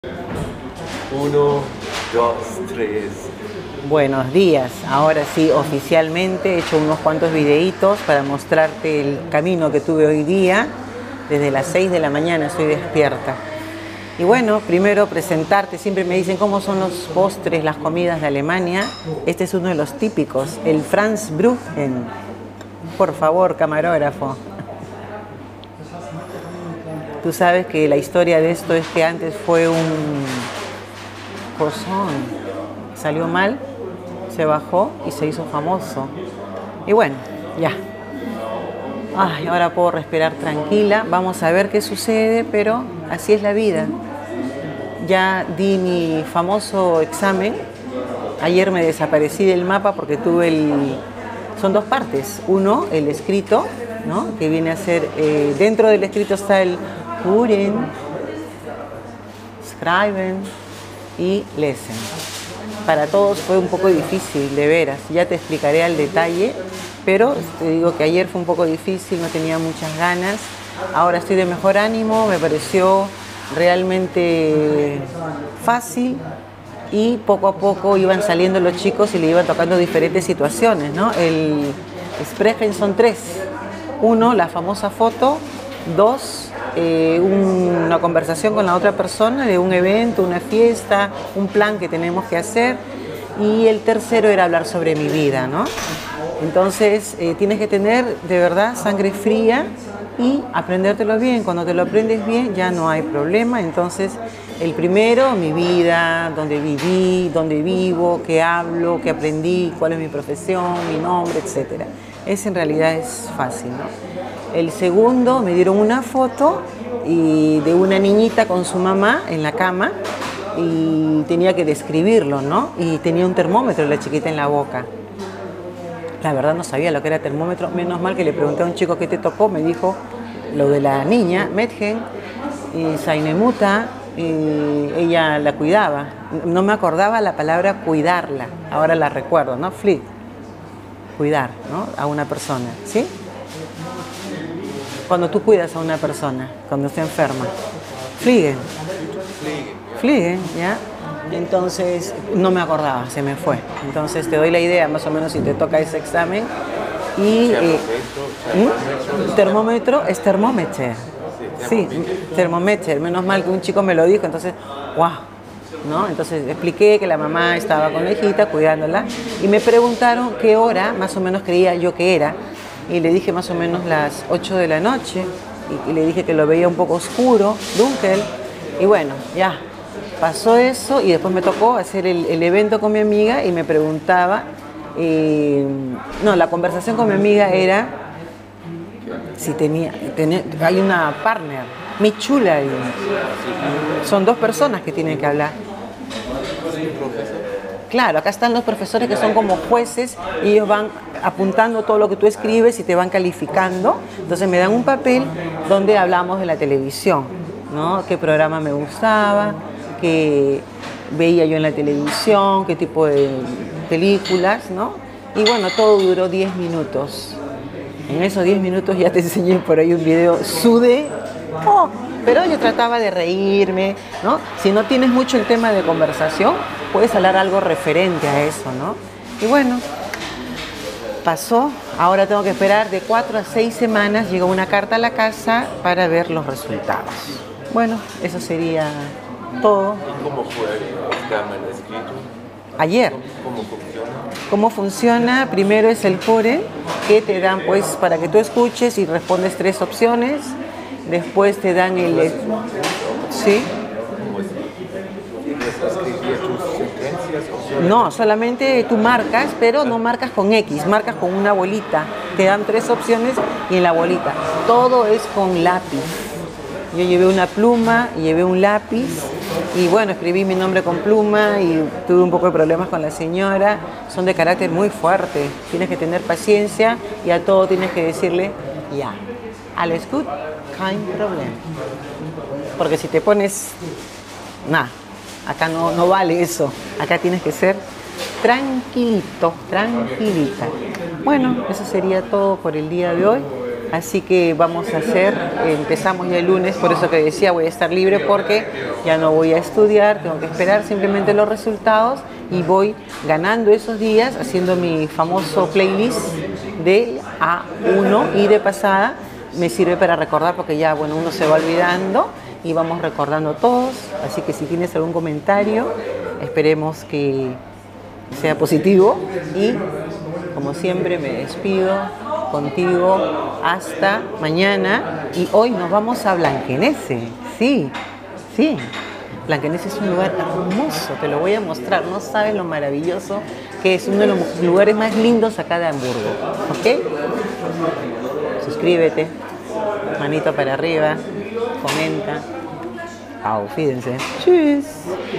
Hice mi Exámen B1 de Alemán por fín!!!
Los extrañaba!!! recién me he recuperado!!!
Estoy bajando varios videos para que veas casi todo paso a paso :)